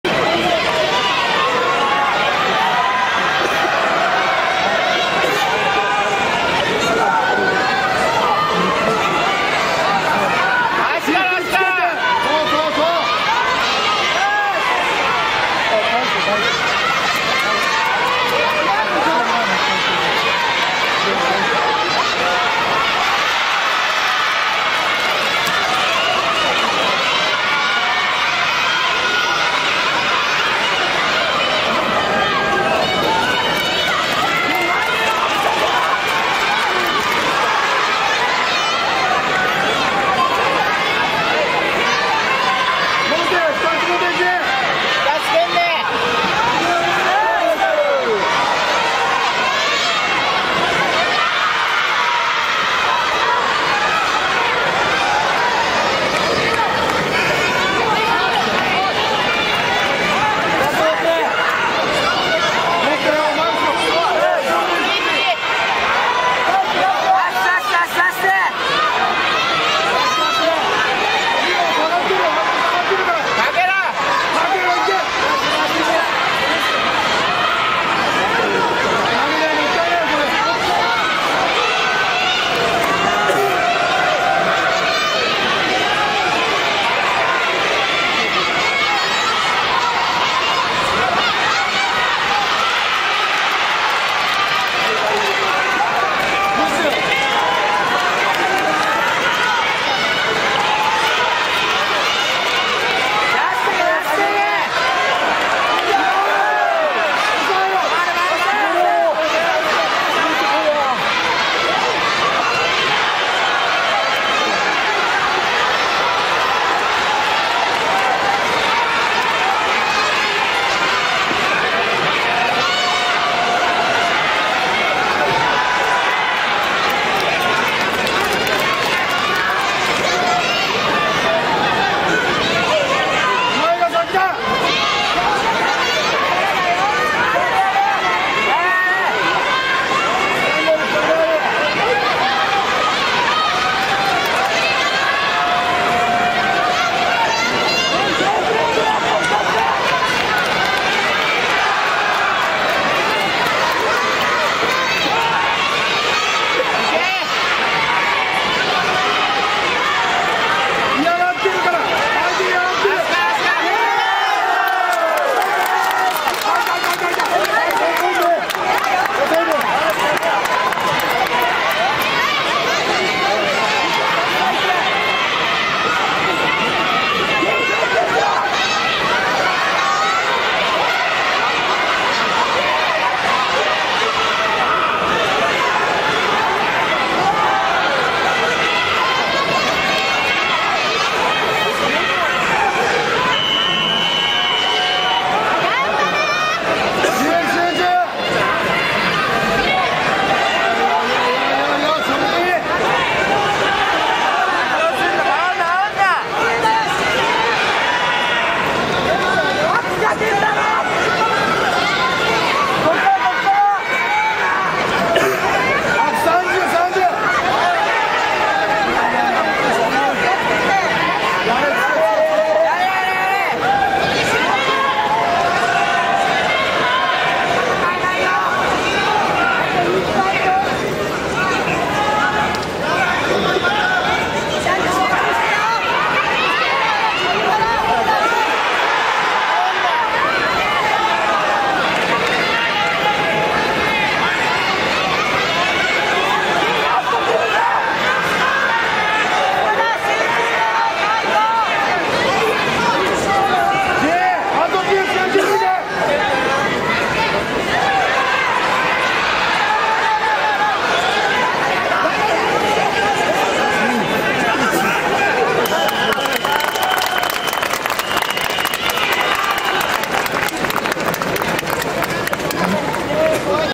準決勝 大成 稲田 対 相模 藤井 2009/3/20 柔道高校選手権
2006年全中超級決勝戦の再現